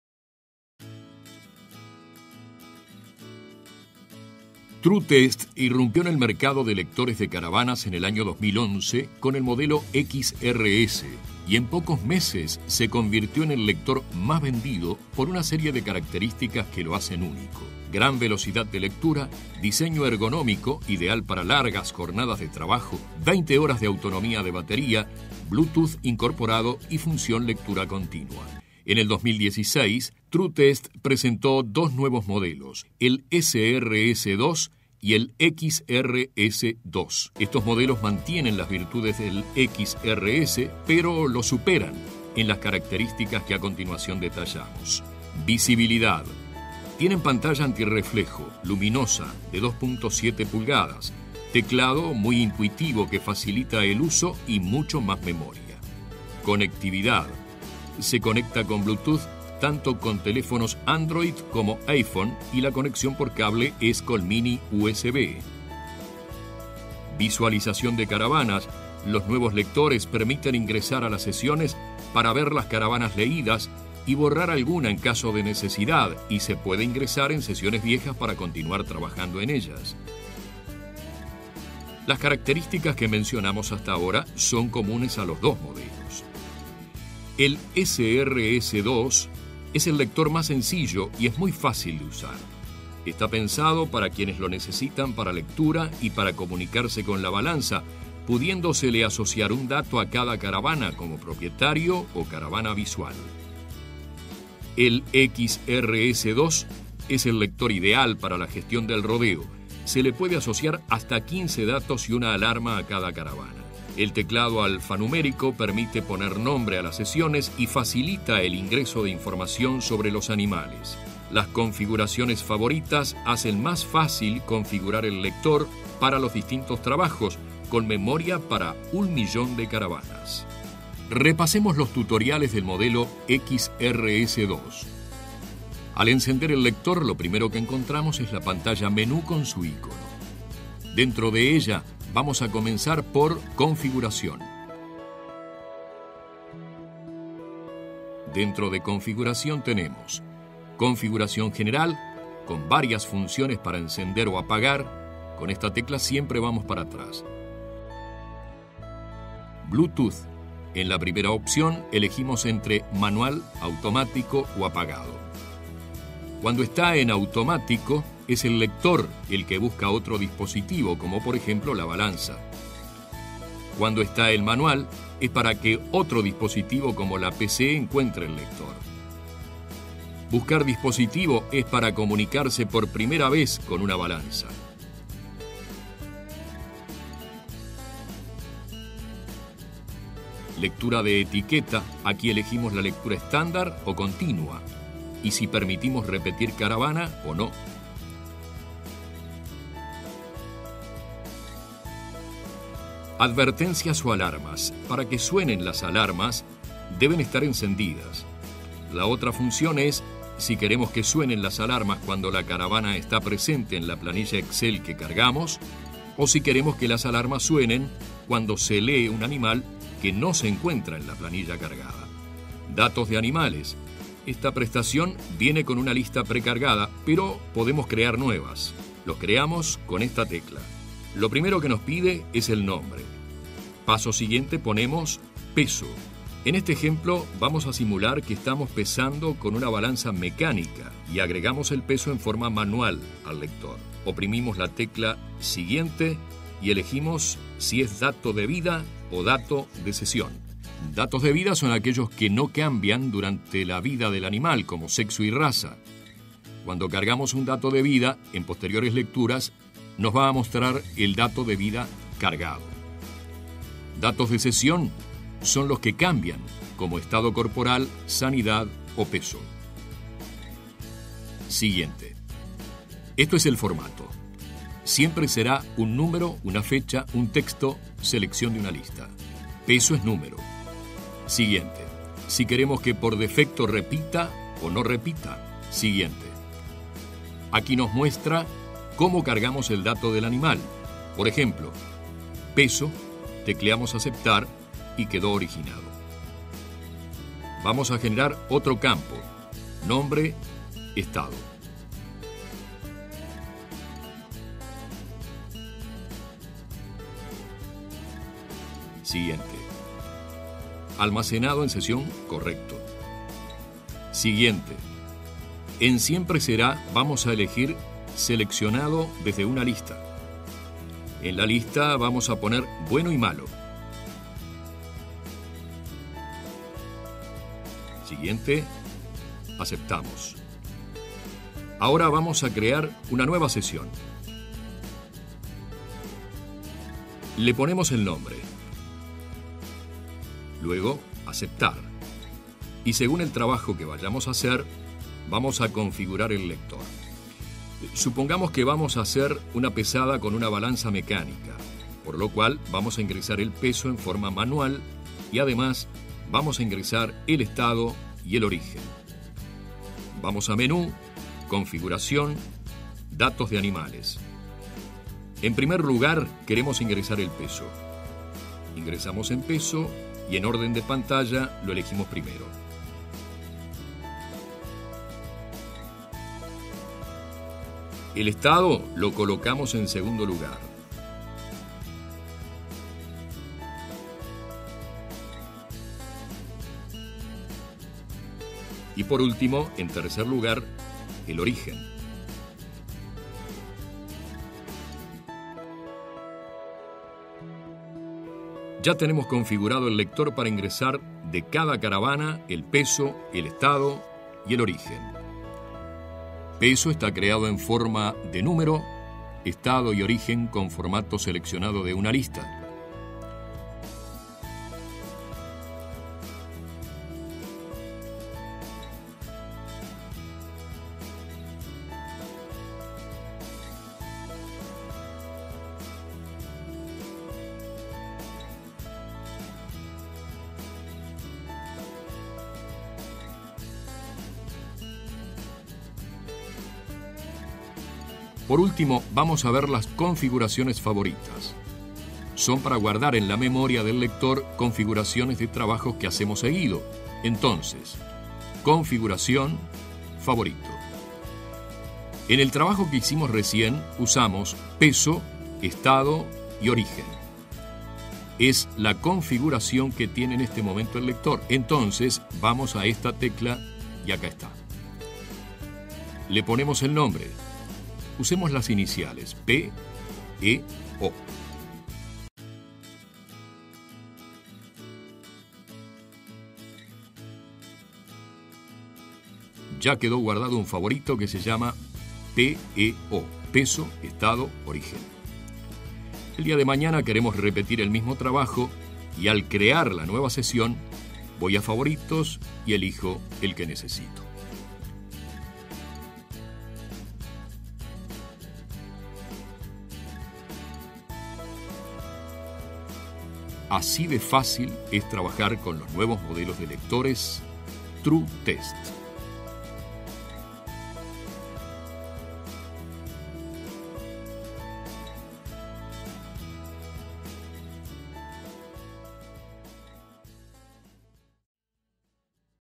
Lector Tru Test XRS2 y SRS2, tutorial
Pantalla visible a la luz solar, fácil lectura.
 Teclado alfanumérico para ingreso de datos.
 Veloz lectura.
 Alertas y 15 campos personalizables.
 Añadir y editar sesiones.
 1Millon de registros.
 Descargar información a Windows, Android o iOS.